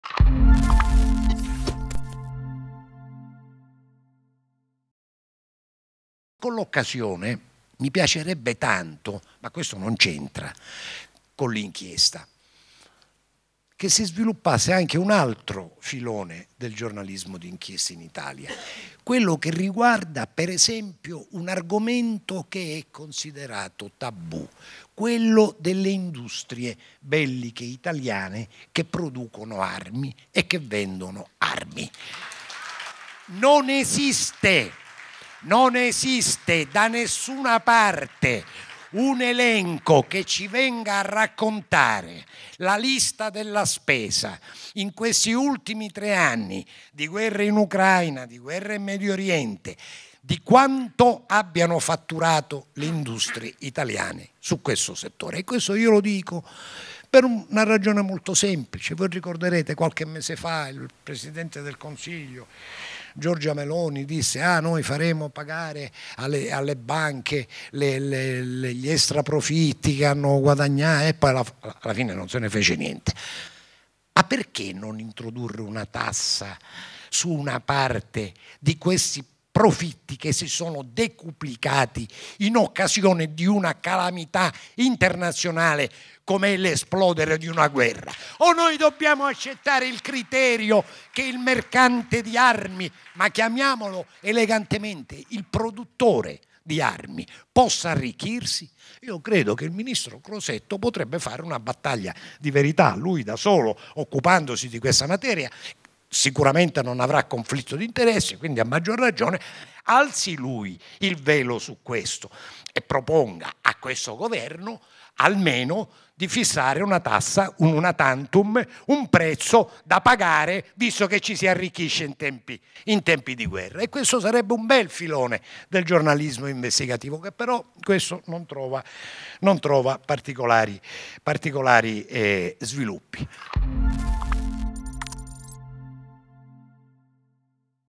Crosetto, quanto stanno guadagnando le industrie italiane che fanno armi?
Mentre Papa Francesco chiede il “coraggio di negoziare” una Pace e torna ad attaccare l’industria delle armi, ricordando a tutti che è sempre dietro ad ogni guerra e si “guadagna per uccidere”. Il nostro Paese, silente, partecipa al banchetto. E allora vale la pena ripetere ciò che ho già detto giovedì, in maniera provocatoria, al Teatro Garbatella di Roma, dove si è tenuta la presentazione del libro "Il Colpo di Spugna" (ed. Fuoriscena), che ho scritto assieme al sostituto procuratore nazionale antimafia Nino Di Matteo.
“Mi piacerebbe che si sviluppasse un altro filone del giornalismo d’inchiesta in Italia, quello che riguarda un argomento considerato tabù.
Quello delle industrie belliche italiane che producono e vendono armi.
Non esiste da nessuna parte un elenco che ci venga a raccontare la lista della spesa in questi ultimi tre anni di guerra in Ucraina e di guerra in Medio Oriente e di quanto abbiano fatturato le industrie italiane in questo settore.
Io lo dico per una ragione molto semplice: qualche mese fa Giorgia Meloni disse che avrebbe fatto pagare alle banche gli extraprofitti. E alla fine non se ne fece niente.
Ma perché non introdurre allora una tassa su questi profitti che si sono decuplicati con la calamità di una esplosione di una guerra?
Crosetto potrebbe fare una battaglia di verità. Alzi lui il velo su questo e proponga a questo governo di fissare una tassa, una-tantum, da pagare. Visto che ci si arricchisce in tempi di guerra.
Ed oggi non lo può nascondere più nessuno.